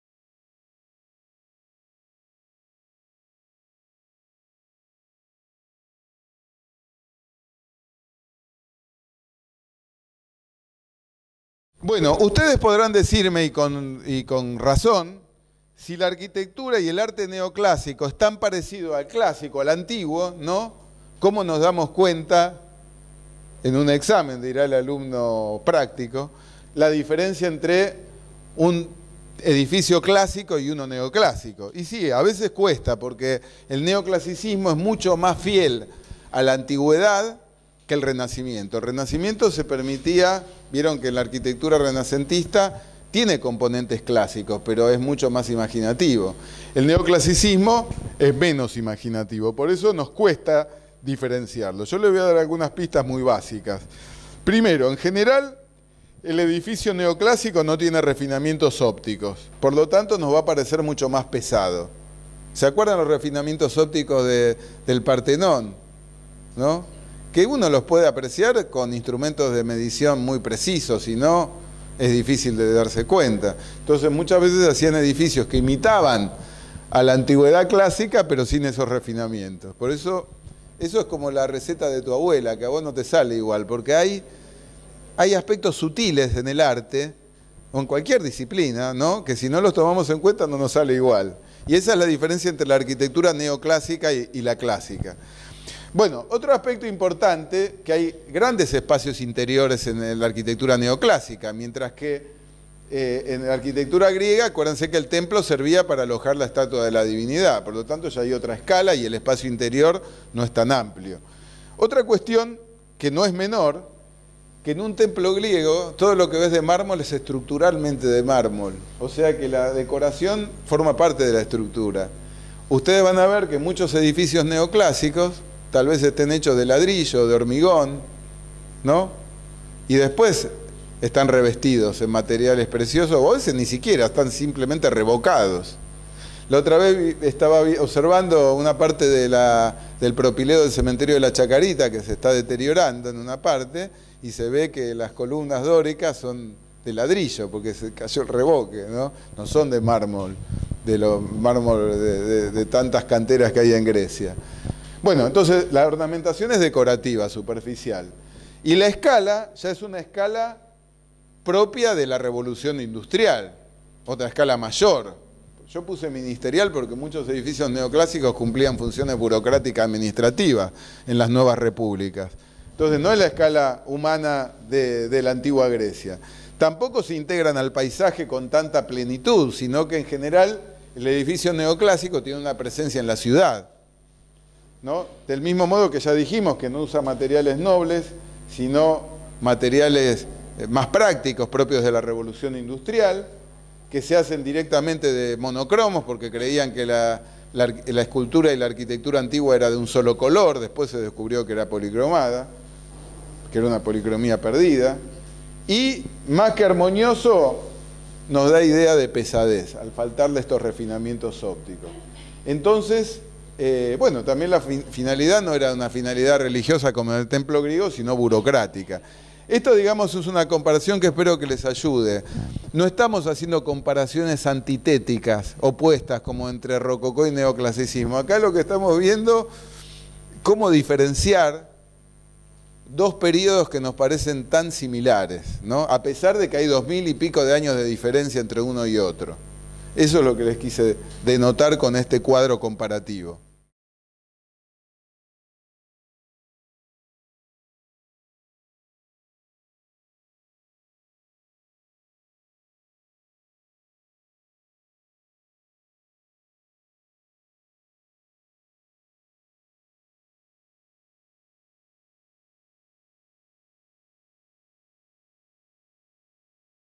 198 ¿CUÁL ES LA DIFERENCIA ENTRE LO CLÁSICO Y LO NEOCLÁSICO? POR EL PROF. JUAN LAZARA
Rococó, Luis XV, Versalles, Neoclasicismo, Napoleón, Schinkel, Romanticismo, Delacroix.